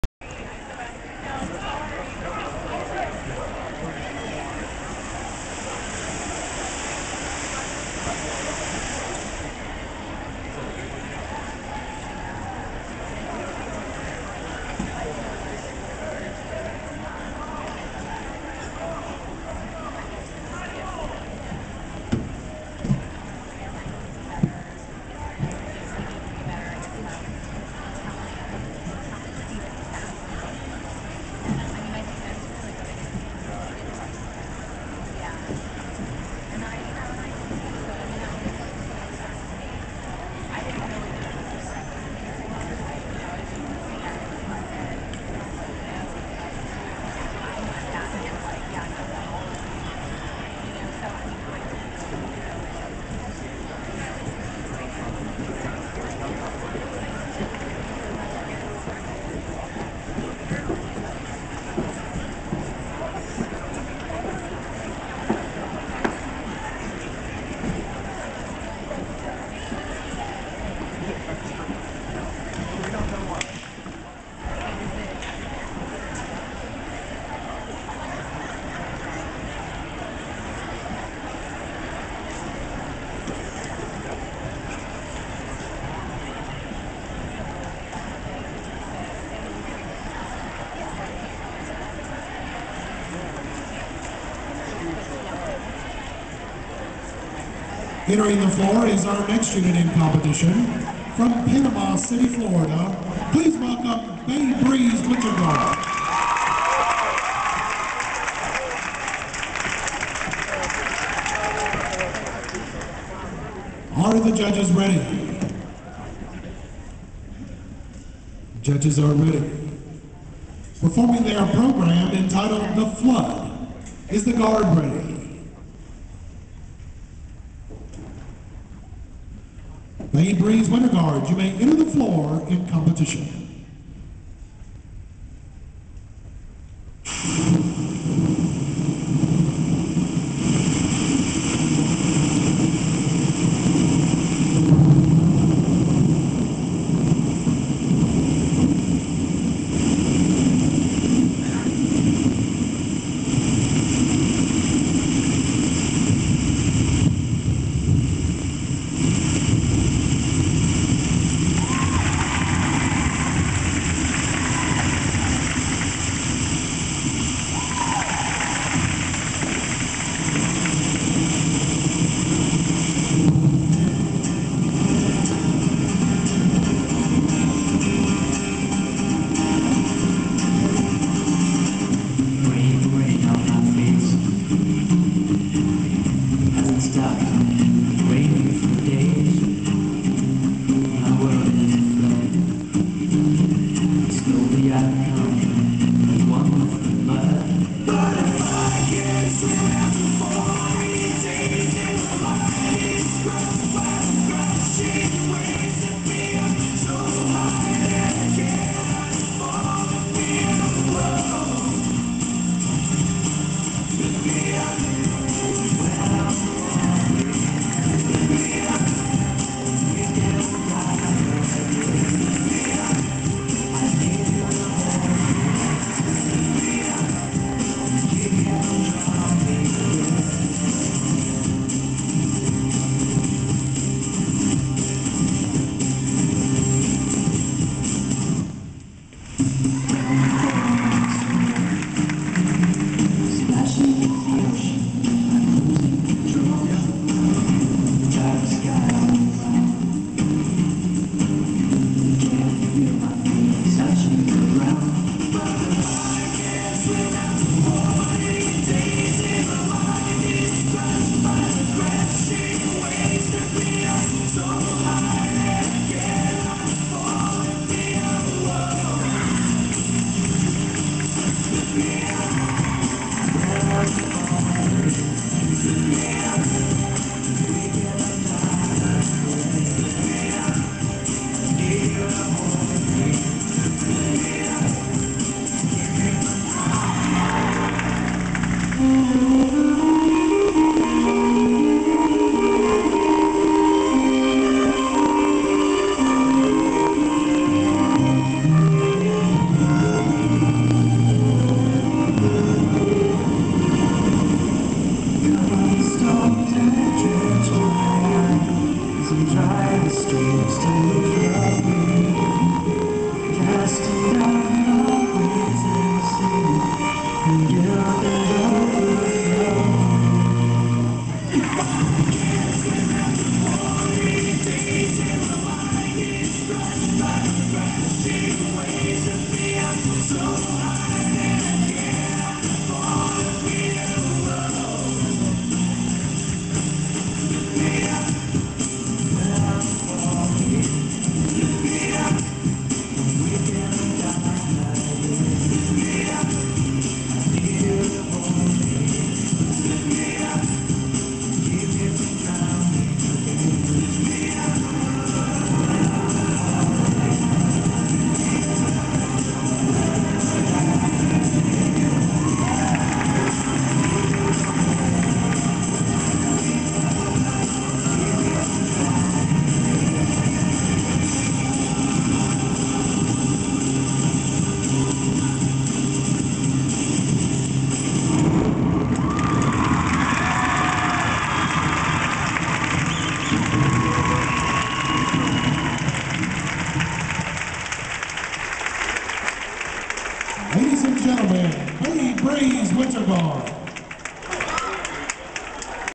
Bay Breeze Championship Show, "The Flood."
The Million Dollar Band presents Bay Breeze Competitive Winterguard @Championships 3/28/09. Tate H.S.

Team Captain is Amanda Hill, Band Director is Nick Efstathiou, Guard Director is Jonathan Soyka. Also under the assistance of Kelly Efstathiou and "The Larry." =]